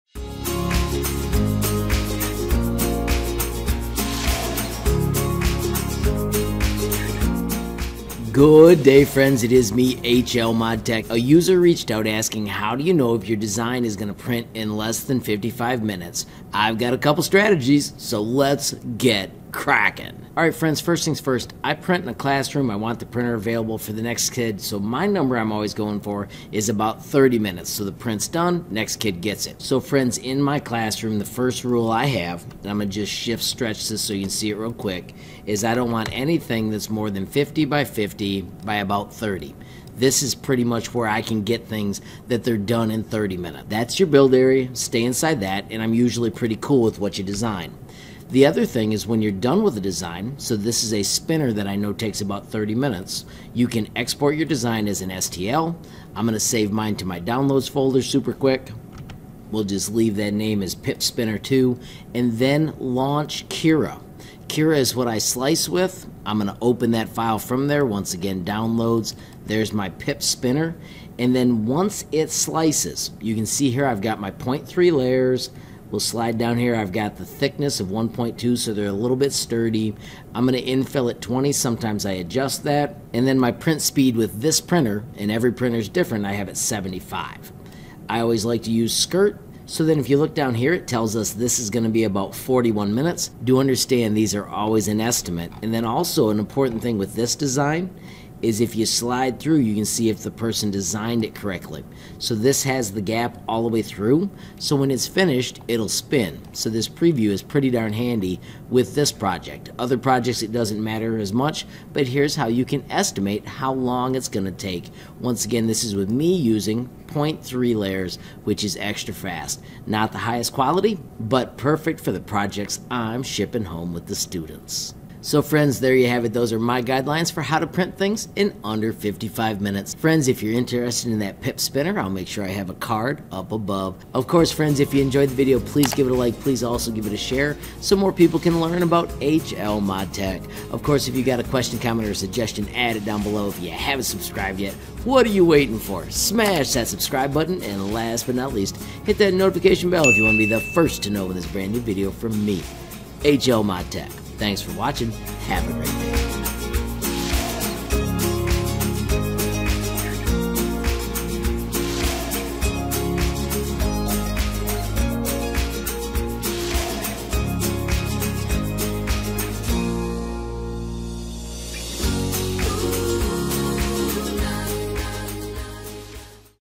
How fast will Cura Print your 3D Design? Simple Steps to check
A user asked how to determine if a print would be less than 55 minutes. I share a few things I use in my classroom.   You will also learn How fast will Cura Print your 3D Design using a few Simple Steps to check.